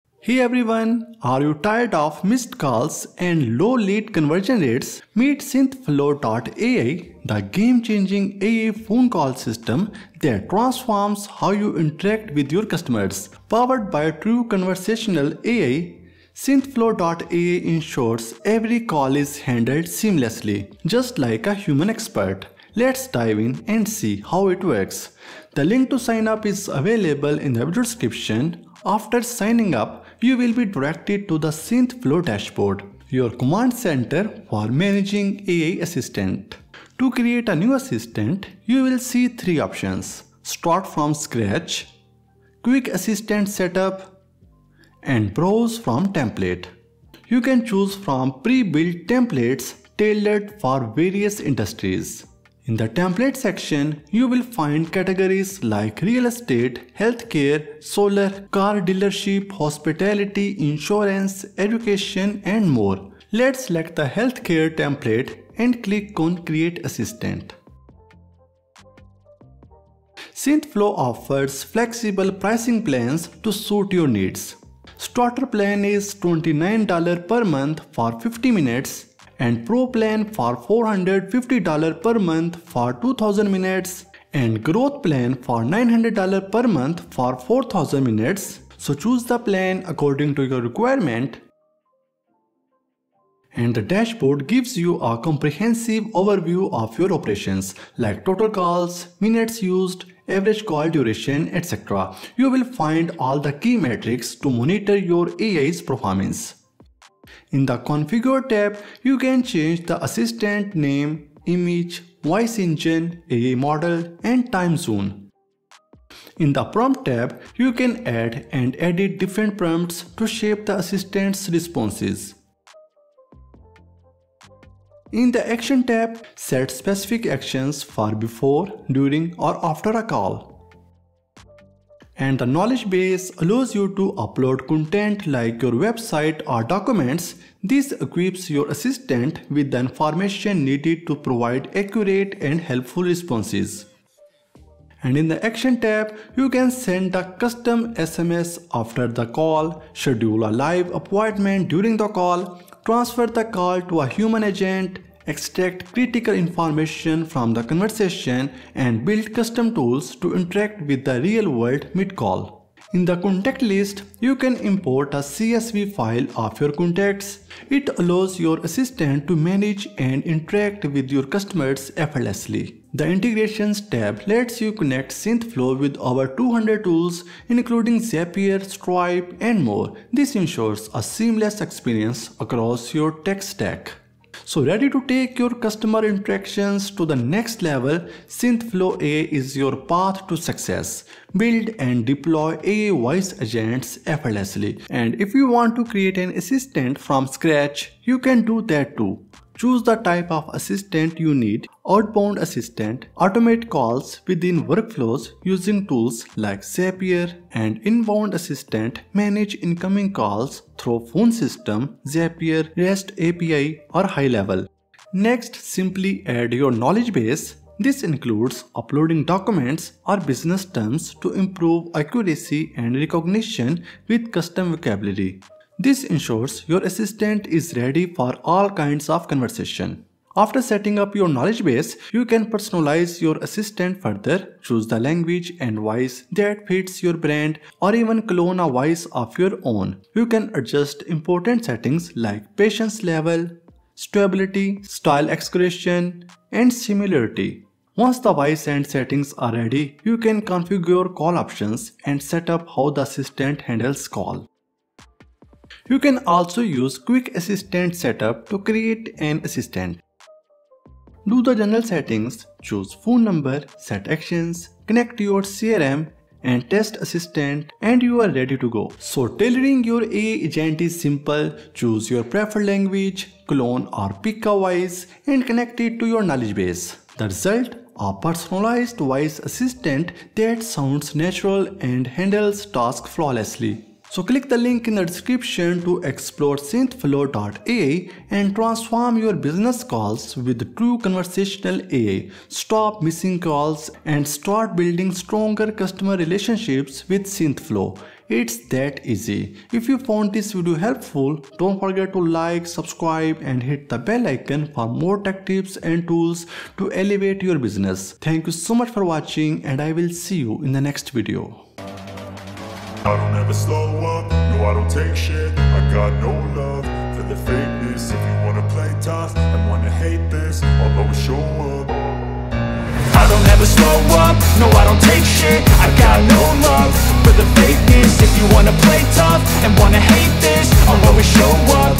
Create AI Phone Assistants in Minutes with SynthFlow AI - Smart AI Voice Agents
How to create AI-powered phone assistants effortlessly with SynthFlow AI.
In this video, we’ll show you how to automate calls in minutes using smart AI voice agents that sound human, handle customer inquiries, book appointments, and more. With no coding required, you’ll streamline workflows, improve customer interactions, and boost business efficiency. Watch now and transform your communication!